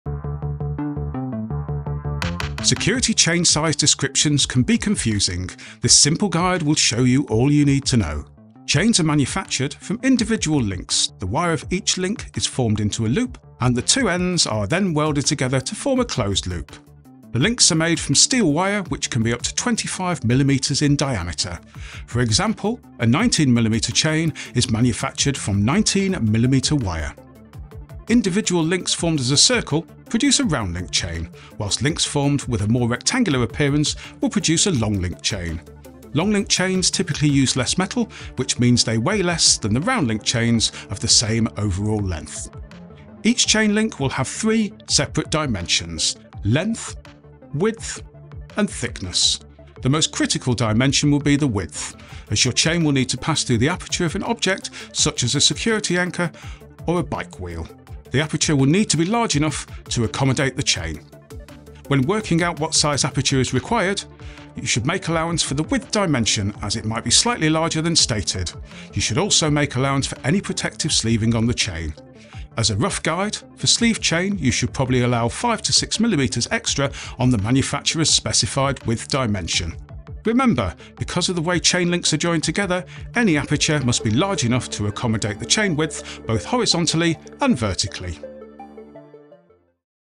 Understanding Security Chain Sizes - a 90 Second Insight video
This 90 Second Insights video explains what chain sizes mean and how you can make sure that when buying your security chain, you will choose the best security chain for your needs.